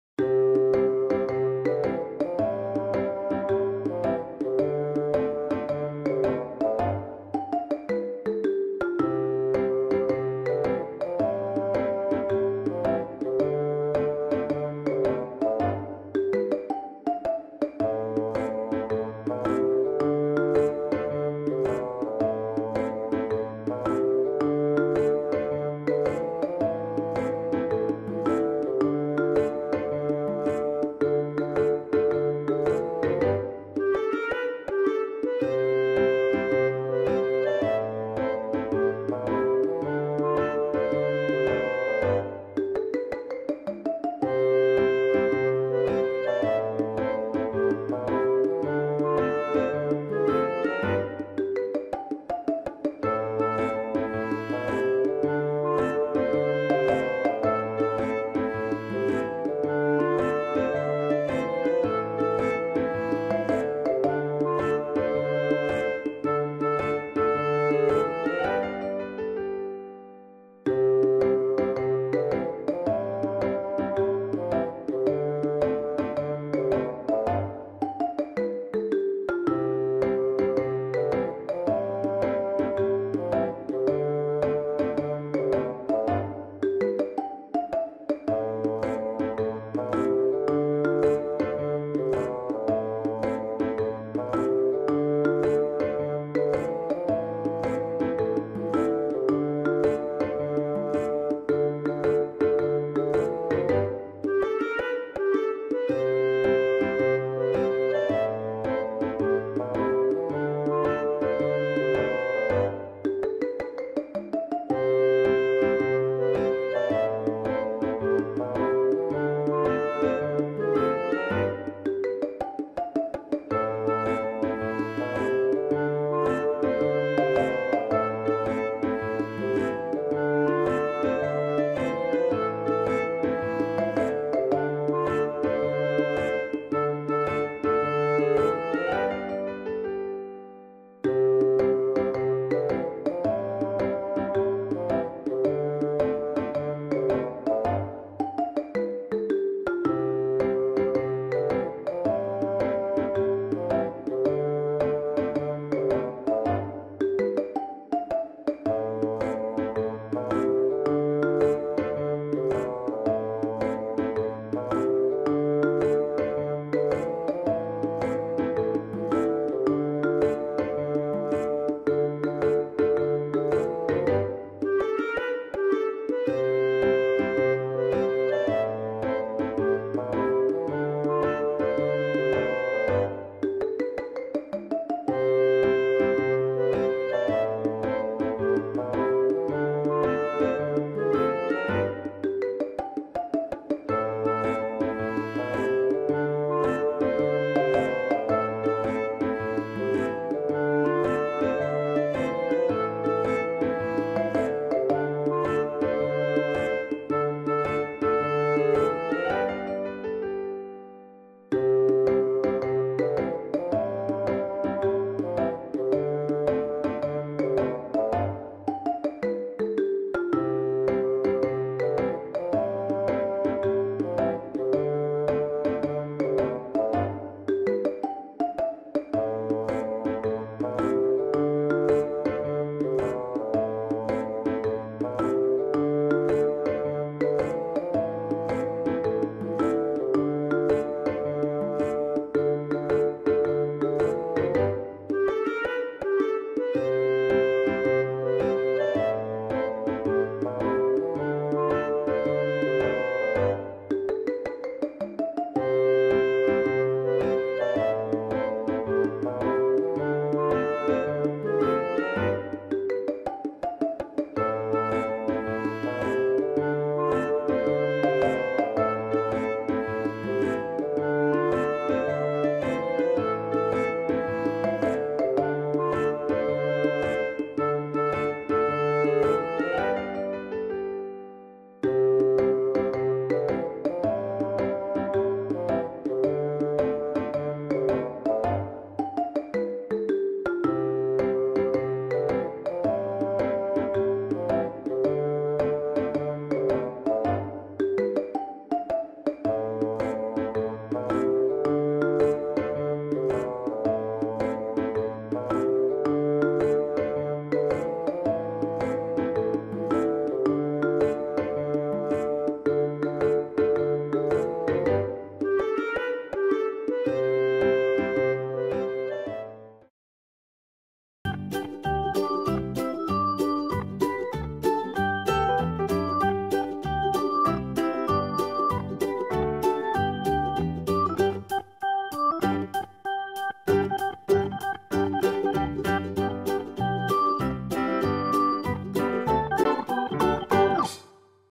【ウソ？ホント！！】すみっコぐらし 新キャラクター作ってみた♪ Needle Felting Sumikkogurashi 角落生物
その名も”ぺんぎん（本物）”
しろくまの友達のようです♪
早速、羊毛フェルトで作ってみました(*^^*)

エイプリルフールだから嘘？？と思った方、、、

本当ですよーーー！！！！